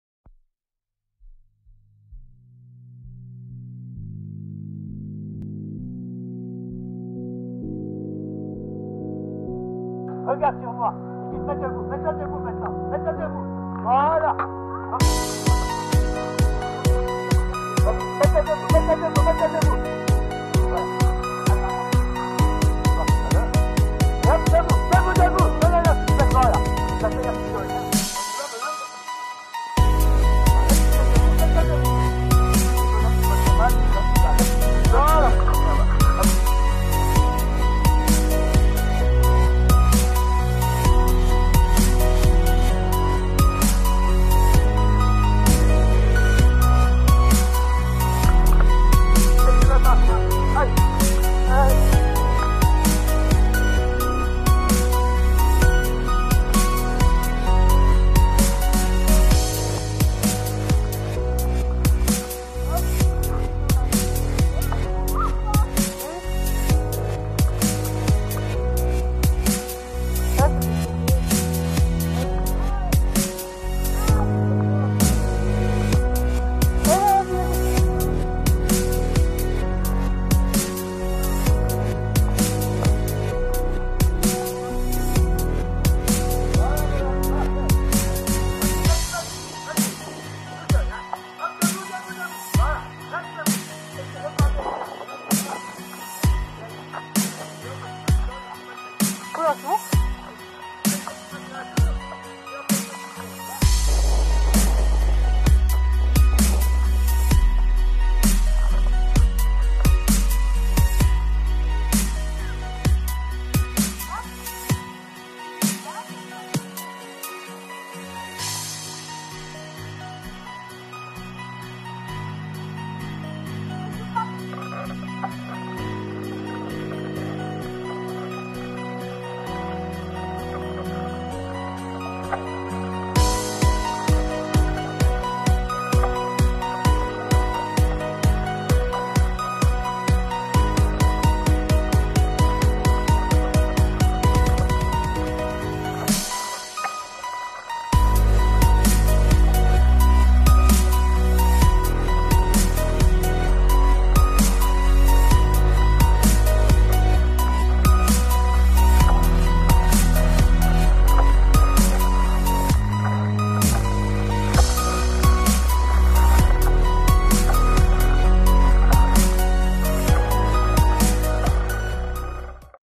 Journée de découverte de VTT 🚵 a l'ASCL Montreux-Vieux
Journée de découverte de VTT par l'ASCL Montreux-Vieux. La journée c'est déroulée le samedi 9 septembre 2017. La journée a été organisée dans le but de faire découvrir le VTT aux plus jeunes qui sont intéressés.